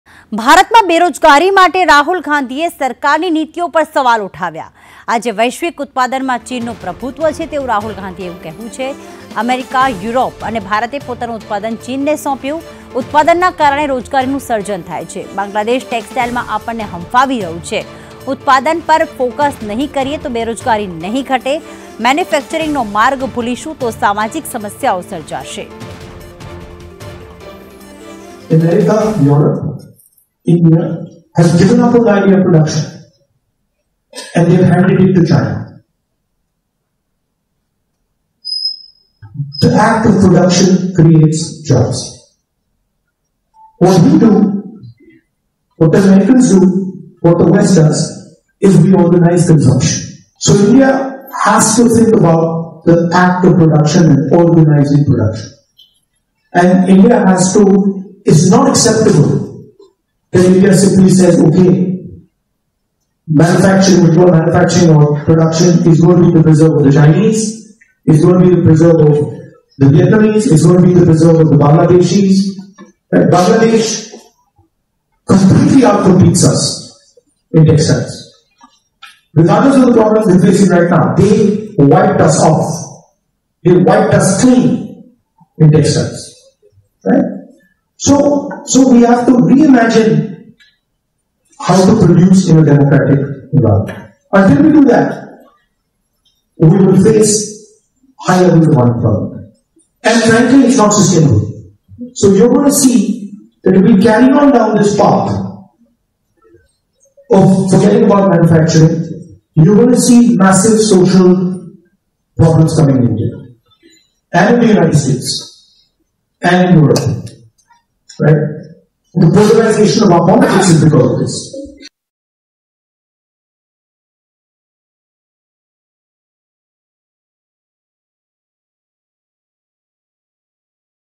Rahul Gandhi | બેરોજગારી માટે રાહુલ ગાંધીએ સરકારની નીતિઓ પર ઉઠાવ્યા સવાલ | Unemployment
ભારતમાં બેરોજગારી માટે રાહુલ ગાંધીએ સરકારની નીતિઓ પર ઉઠાવ્યા સવાલ
આજે વૈશ્વિક ઉત્પાદનમાં ચીનનું પ્રભુત્વઃ રાહુલ
અમેરિકા, યુરોપ અને ભારતે પોતાનું ઉત્પાદન ચીનને સોંપ્યુઃ રાહુલ
ઉત્પાદનને કારણે રોજગારીનું સર્જન થાય છેઃ રાહુલ
બાંગ્લાદેશ ટેક્સટાઈલમાં આપણને હંફાવી રહ્યું છેઃ રાહુલ
ઉત્પાદન પર ફોકસ નહીં કરીએ તો બેરોજગારી નહીં ઘટેઃ રાહુલ
મેન્યુફેકચરિંગનો માર્ગ ભૂલીશું તો સામાજિક સમસ્યાઓ સર્જાશેઃ રાહુલ